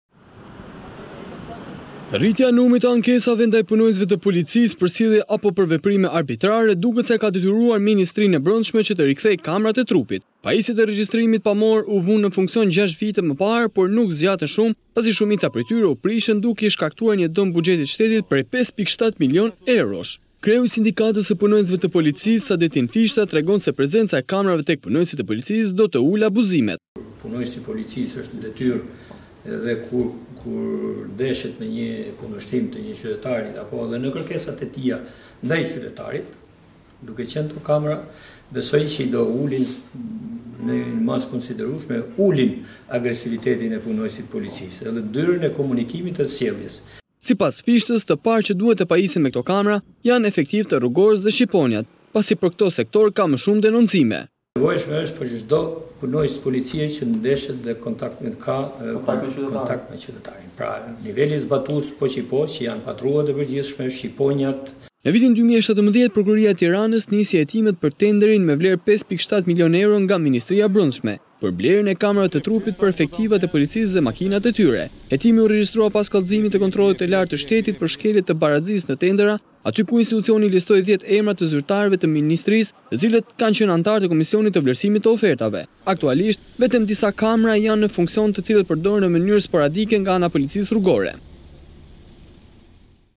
Kamera në trupin e policëve për të ulur abuzimet, Fishta: Vendim i vonuar
Kamerat e trupit të punonjësve të policisë do të rivendosen në funksion ndërkohë që ky proces do të kushtojë 60 milion lekë në buxhetin e shtetit. Kamerat e blera me tenderin e parë kanë dalë shumica jashtë funksionit. ndërkohë që ekspertët mendojnë se kjo nisme e Ministrisë është e vonuar pasi shumë ngjarje të ndodhura me protagoniste policinë nuk po dokumentohen për shkak të mungesës së kamerave të trupit.

Rritja e numrit të ankesave ndaj punojësve të policisë për sjellje apo për veprime arbitrare duket se e ka detyruar Ministrinë e Brendshme që të rikthejë kamerat e trupit.

Pajisjet e regjistrimit pamor u vunë në funksion edhe 6 vite më parë, por nuk zgjatën shumë pasi shumica prej tyre u prishën duke i shkaktuar një dëm buxhetit të shtetit prej 5.7 milion eurosh.

Kreu i Sindikatës së Punonjësve të Policisë, Sadetin Fishta, tregon se prezenca e kamerave tek punonjësit e policisë do të ulë abuzimet.

"Kur punonjësi i policisë është në detyrë dhe kur ndeshet me një kundërshtim të një qytetari apo dhe kërkesat e tij ndaj qytetarit, duke qenë këto kamera besoj që do ulin në masë të konsiderueshme agresivitetin e punonjësit të policisë dhe do ndryshojnë mënyrën e komunikimit dhe sjelljes."

Sipas Fishtës, të parët që duhet të paisjen më këto kamera janë efektivët e Rrugores dhe Shqiponjat, pasi për këto sektorë ka më shumë denoncime.

"Është e nevojshme për çdo polic që ka kontakt me qytetarin,që janë patrullat e përgjithshme, Shqiponjat".

Në vitin 2017, Prokuroria e Tiranës nisi hetimet për tenderin me vlerë 5.7 milionë euro nga Ministria e Brendshme, për blerjen e kamerave të trupit për efektivët e policisë dhe makinat e tyre.

Hetimi u regjistrua pas kallëzimit të Kontrollit të Lartë të Shtetit, për shkelje të barazisë në tendera ku ky institucion listoi 10 emra të zyrtarëvë të Ministrisë, të cilët kanë qenë anëtarë në Komisionin e Vlerësimit të Oferatve.

Aktualisht vetëm disa kamera janë në funksion, të cilat përdoren në mënyrë sporadike nga Policia Rrugore.